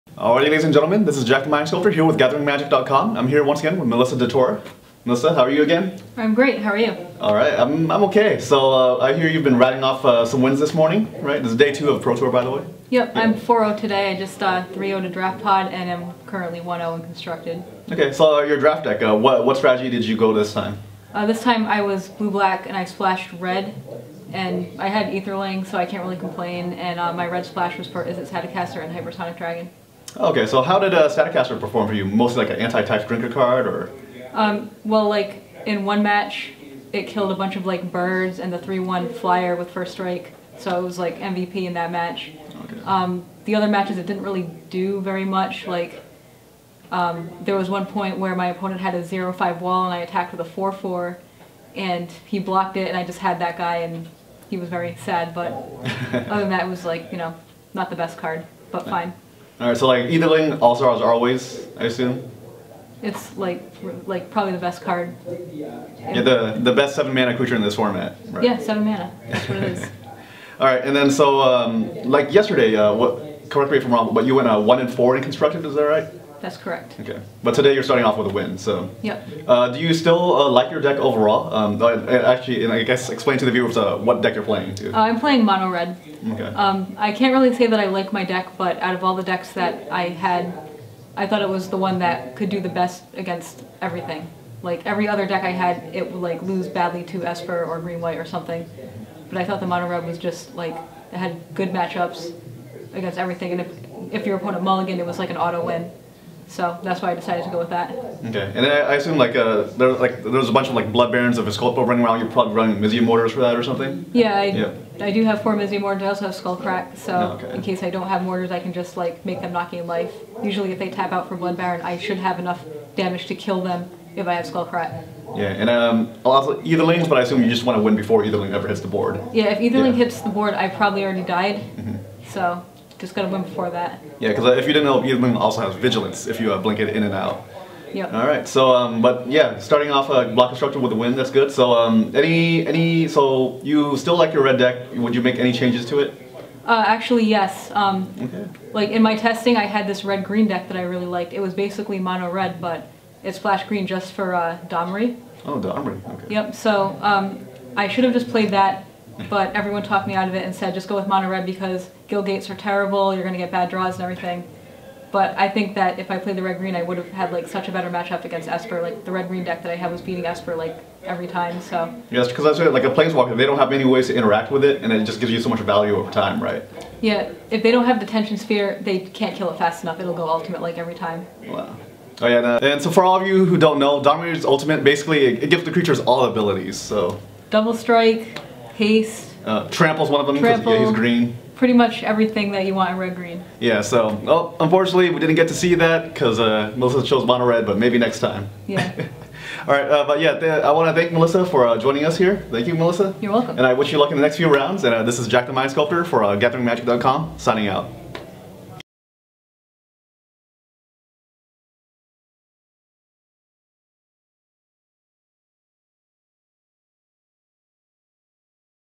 Pro Tour DGM - Melissa DeTora Interview Update
Jack the Mind Sculptor here, sitting down with an update for Melissa DeTora. She talks about her drafts and her block deck.